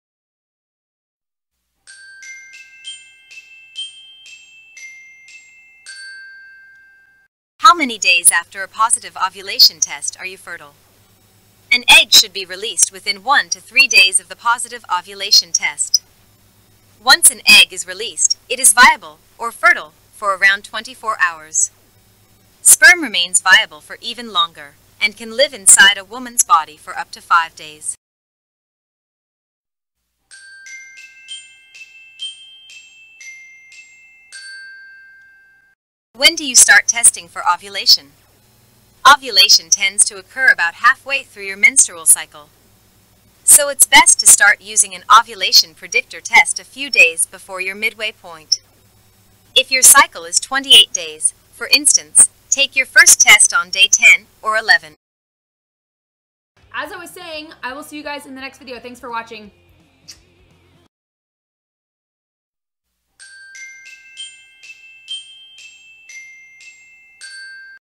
How many days after a positive ovulation test are you fertile? When do you start testing for
How many days after a positive ovulation test are you fertile?   When do you start testing for ovulation?

Why was my OPK test positive for 3 days in a row? Shouldn't it just be positive for 1 day?
Get Pregnant Faster: ovulation tests are the best method for getting pregnant fast | Live OPK Q+A